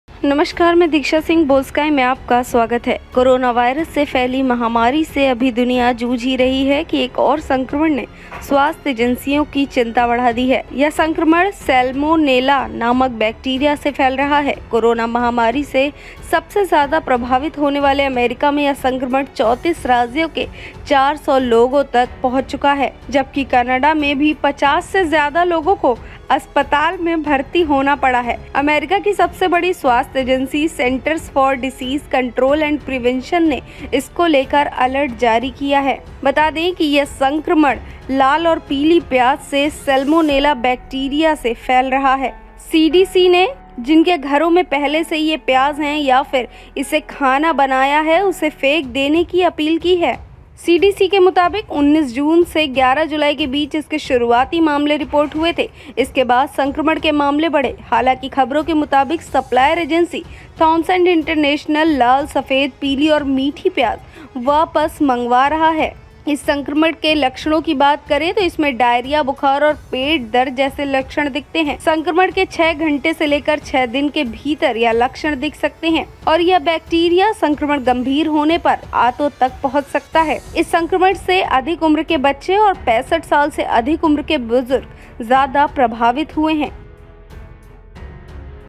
Corona Virus के बाद अब फैला नया Virus, प्याज से फैल रहा है ये संक्रमण |salmonella Virus | Boldsky
The world is still struggling with the corona virus epidemic that another infection has raised the concern of health agencies. This infection is spread by a bacterium called salmonella. The infection has reached 400 people in 34 states in the United States most affected by the Corona epidemic, while more than 50 people have been hospitalized in Canada. American's largest health agency Centers for Disease Control and Prevention (CDC) has issued an alert regarding this.

कोरोना वायरस से फैली महामारी से अभी दुनिया जूझ ही रही है कि एक और संक्रमण ने स्वास्थ्य एजेंसियों की चिंता बढ़ा दी है। यह संक्रमण सैल्मोनेला नामक बैक्टीरिया से फैल रहा है। कोरोना महामारी से सबसे ज्यादा प्रभावित होने वाले अमेरिका में यह संक्रमण 34 राज्यों के 400 लोगों तक पहुंच चुका है, जबकि कनाडा में भी 50 से ज्यादा लोगों को अस्पताल में भर्ती होना पड़ा है। अमेरिकी की सबसे बड़ी स्वास्थ्य एजेंसी सेंटर्स फॉर डिसीज कंट्रोल एंड प्रिवेंशन (सीडीसी) ने इसको लेकर अलर्ट जारी किया है।